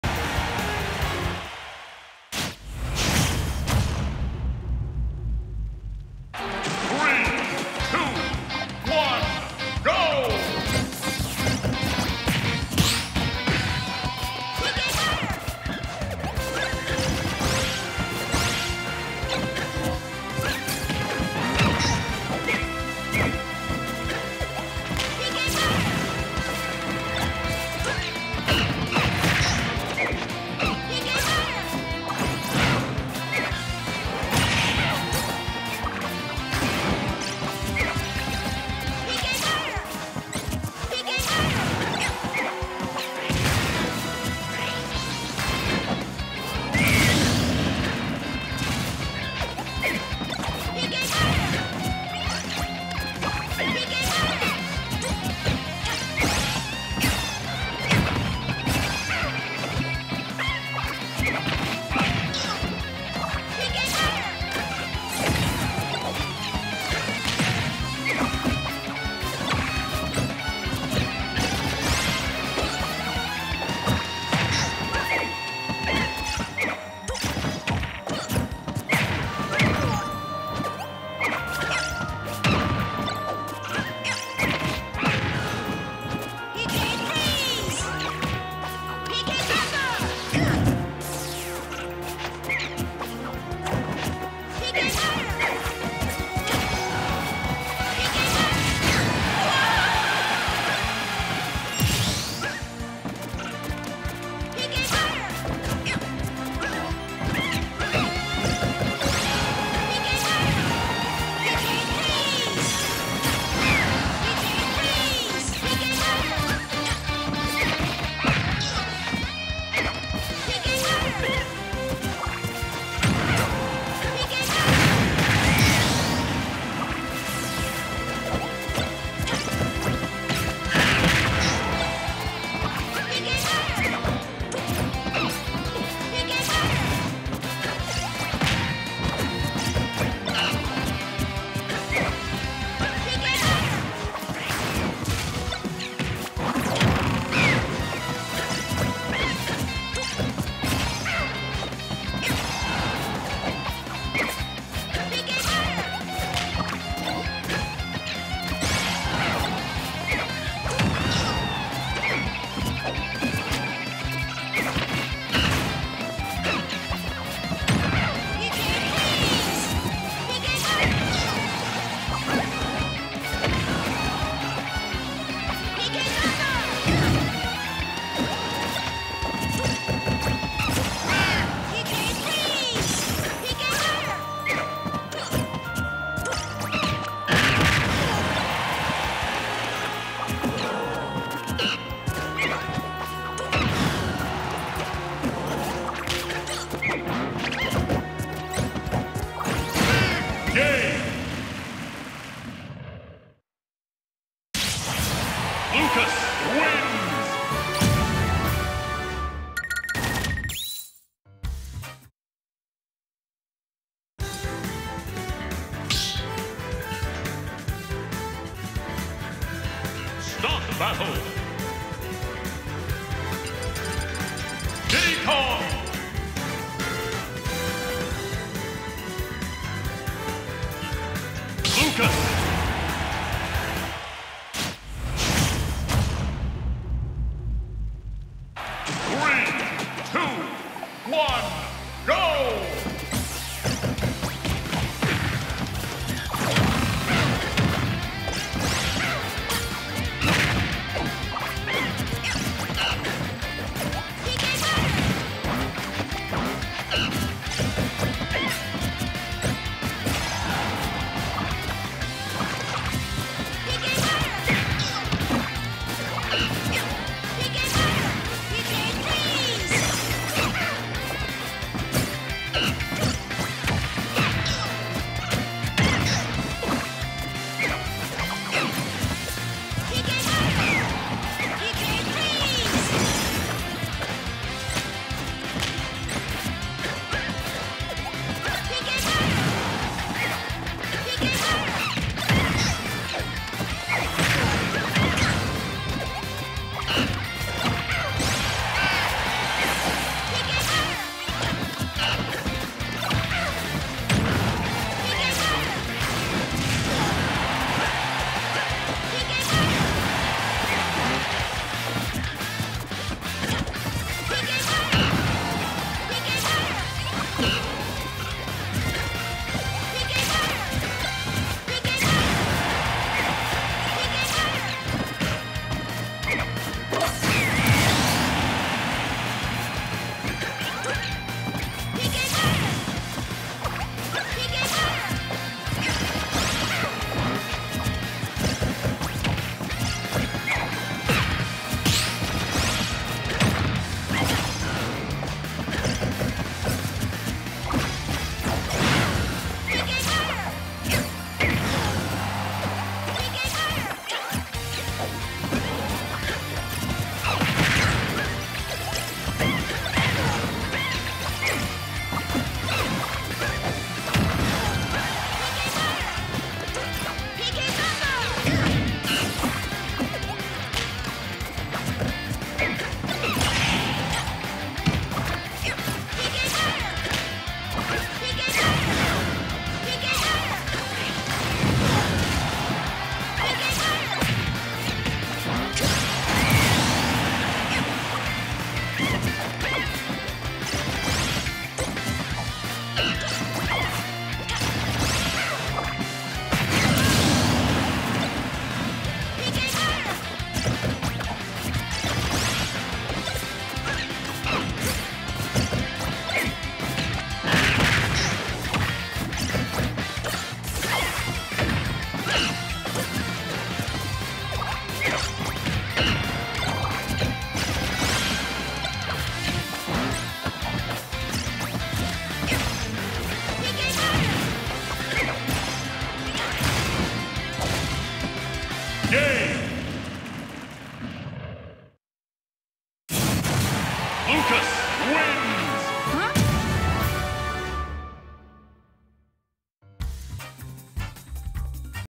Sisslor (Diddy Kong) vs LOST | Sui (Lucas) - Nice Back Air E
Powered by Team Mana and Smashistan

Nice Back Air E
May 21st, 2022
9 شارع ند الحمر - دبي - الإمارات العربية المتحدة